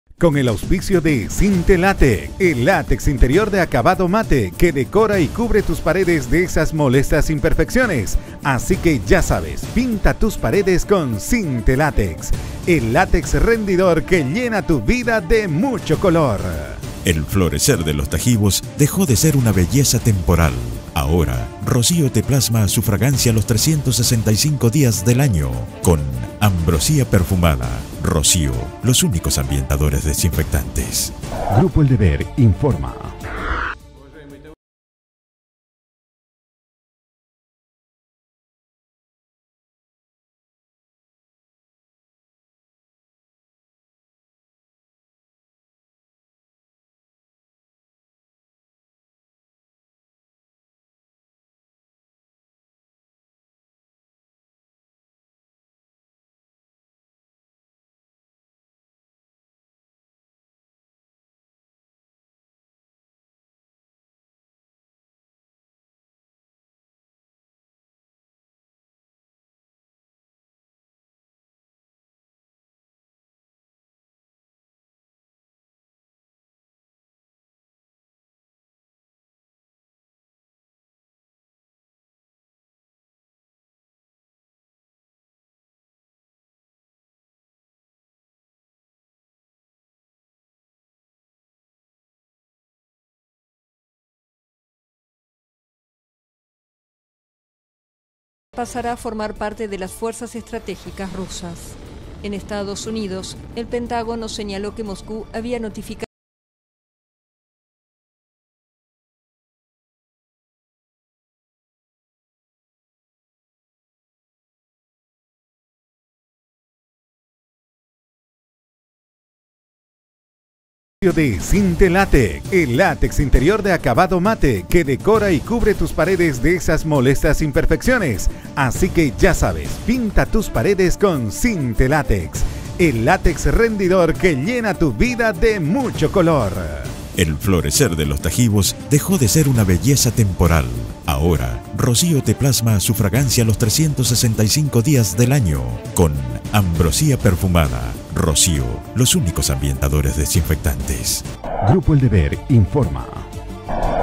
RUSIA ENSAYA MISIL BALÍSTICO INTERCONTINENTAL
Rusia ensaya con éxito un nuevo misil balístico intercontinental